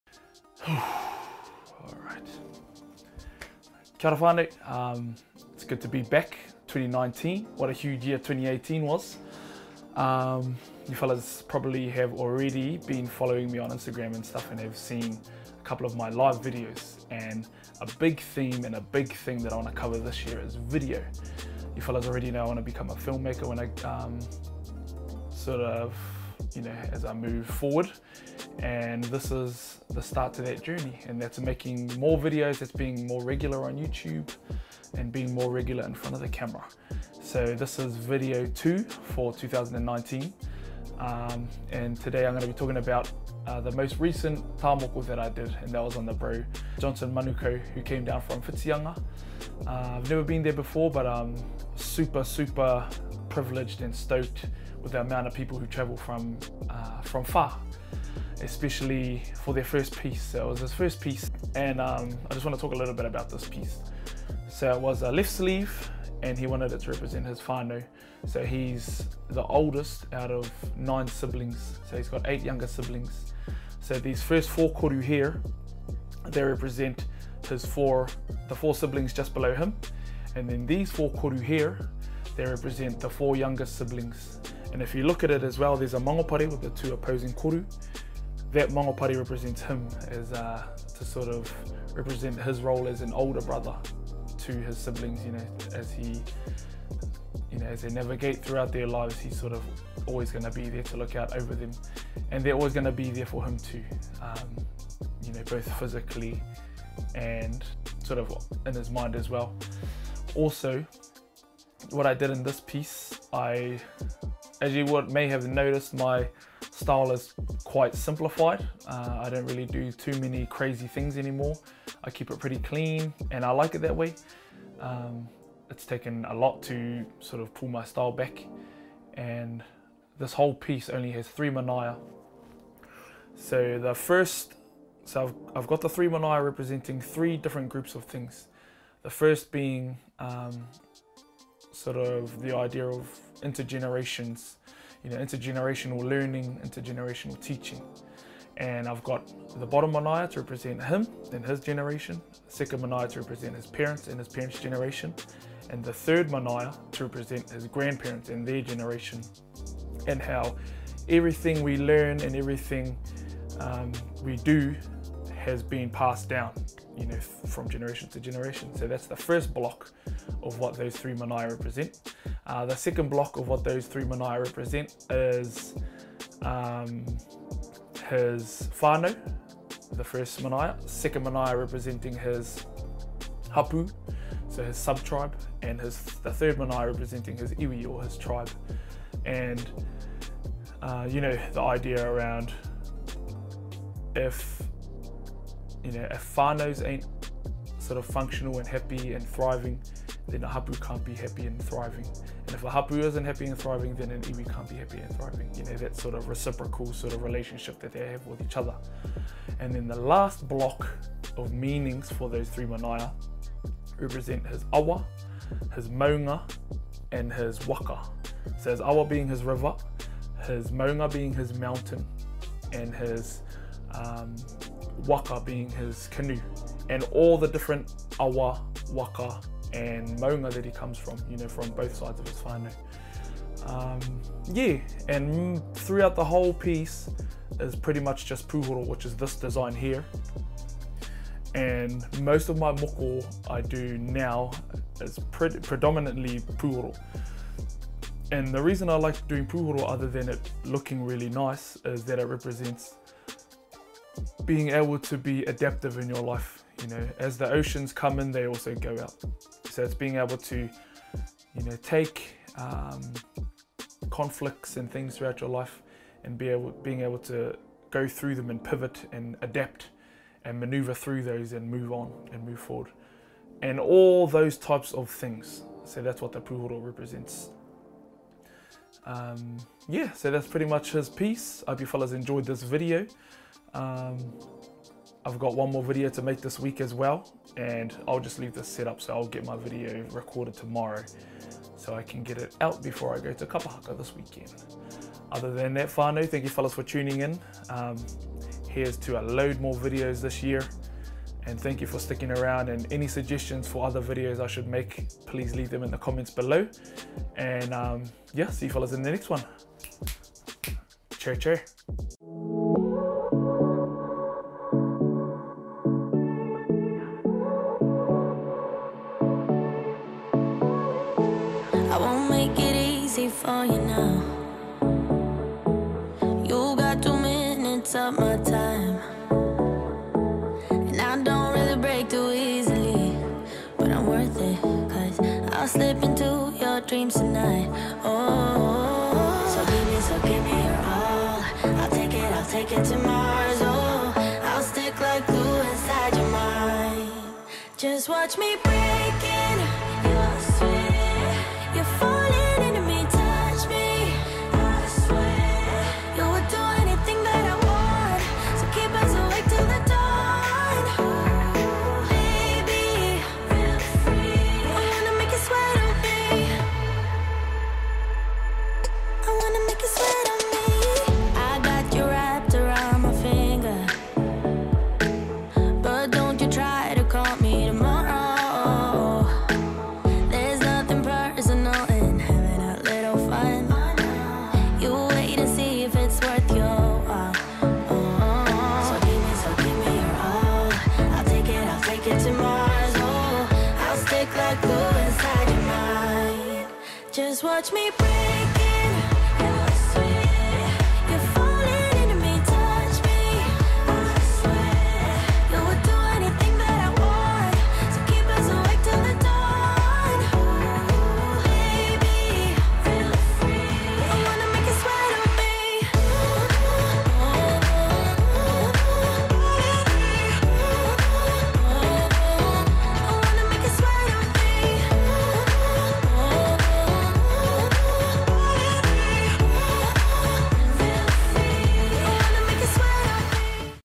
moko walkthrough 2 Johnson Manukau
My second video for the year and second moko walkthrough video. Really happy how this video turned out and I've already learnt a lot since the last walkthrough and learnt a lot through the process of making this video too! I'm looking forward to creating more videos like this.